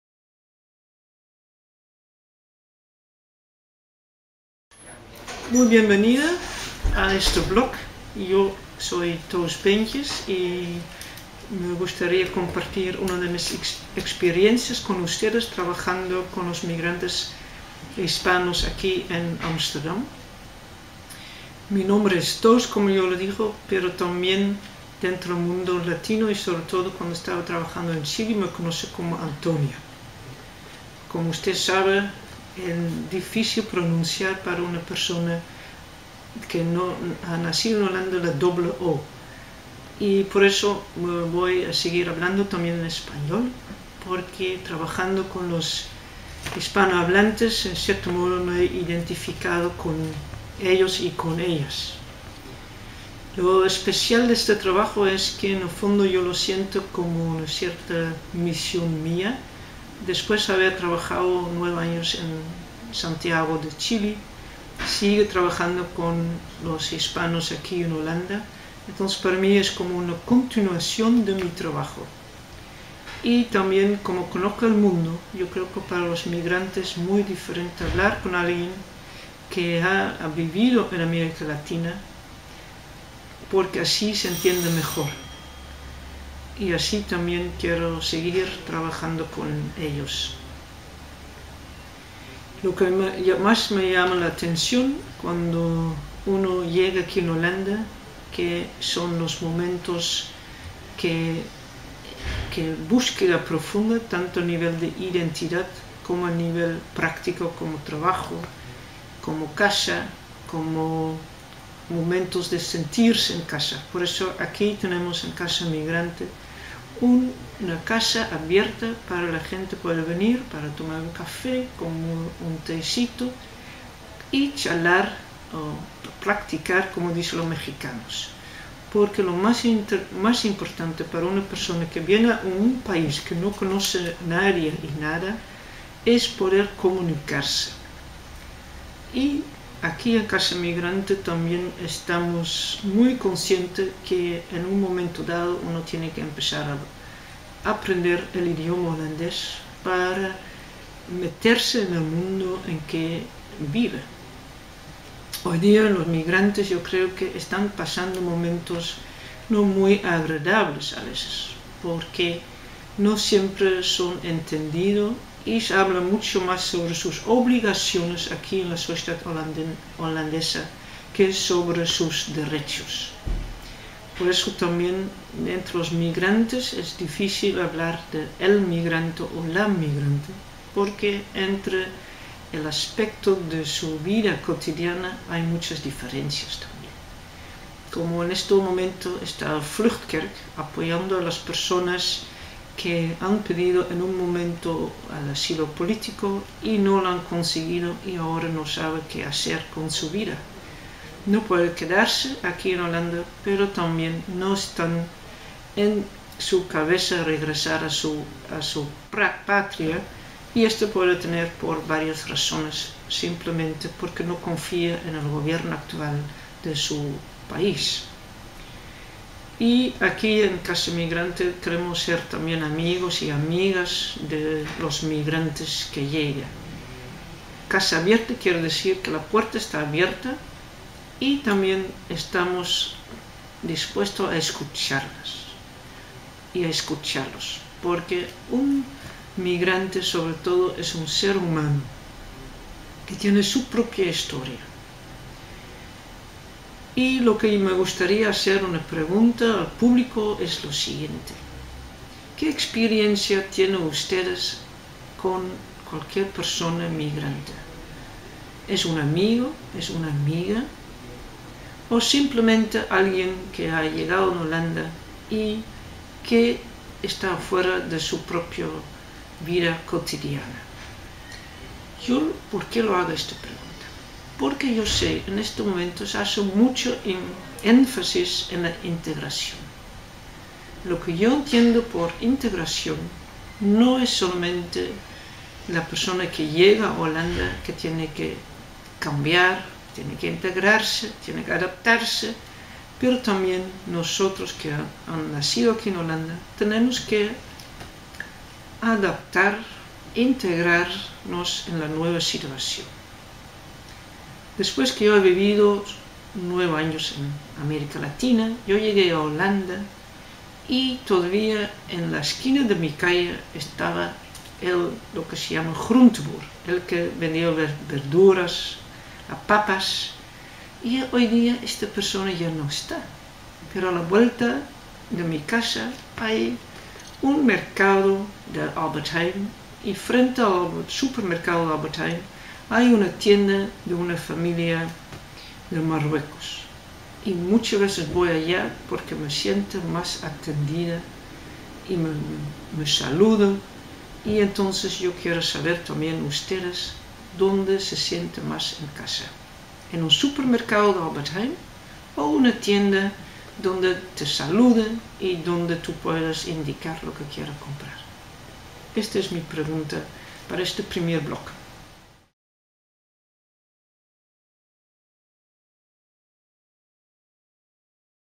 Toos - A life for Hispanic migrants Vlog I
Video blog I for LA Ruta & 'Migratie in Beeld' of Toos Bentjes

After living in Santiago de Chile in the 1970's and 80's Toos came
back to Amsterdam to work with Hispanic migrants at Casa Migrante. In her
first video blog she describes what makes it special for her to work with
them and what she thinks about the current situation of migrants living in
the Netherlands.

Share your experience with her, post your video blog!